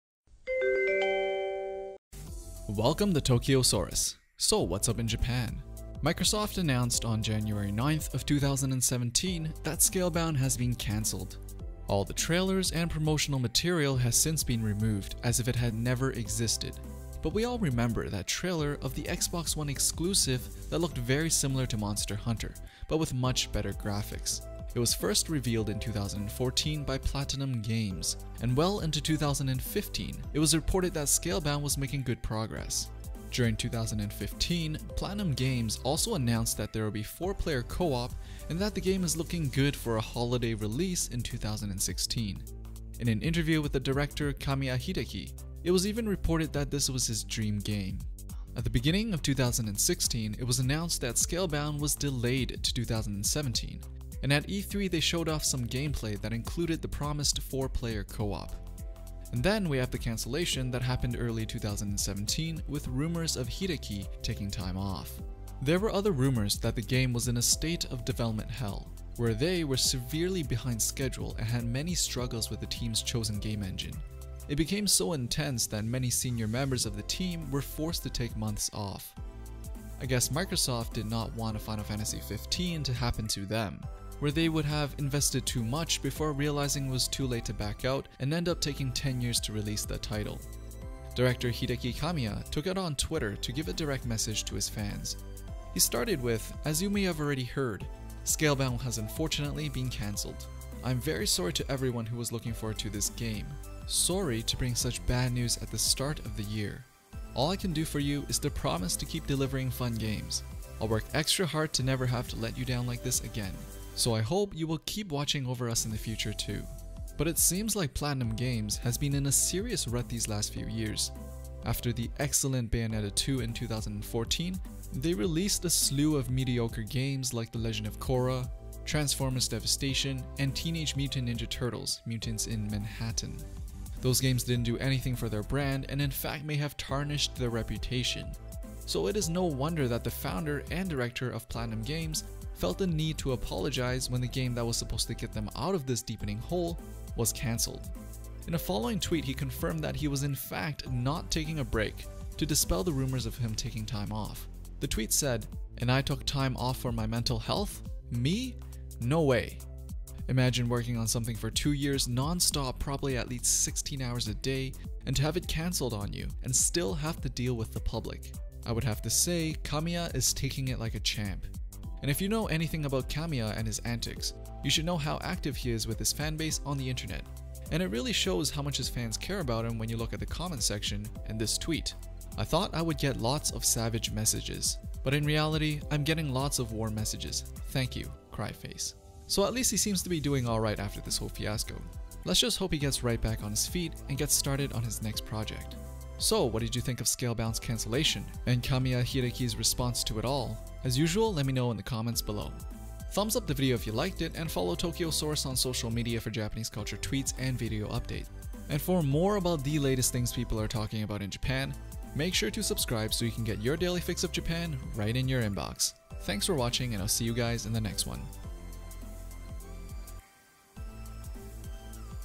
Kamiya Hideki (Scalebound Director) Responds to Cancellation
After the cancellation of Scalebound, PlatinumGames founder and director Kamiya Hideki took it to twitter to respond to the whole event. Microsoft cancelled Scalebound on January 9th 2017 and after what seemed like many years of hard work, suddenly the project was cut. Nobody knows the exact reason but it seemed like they were having trouble in the game engine as well management issues with senior team members taking forced breaks. It was looking like another Final Fantasy XV and Microsoft was going to have none of that. So what does Kamiya Hideki has to say about all this on twitter?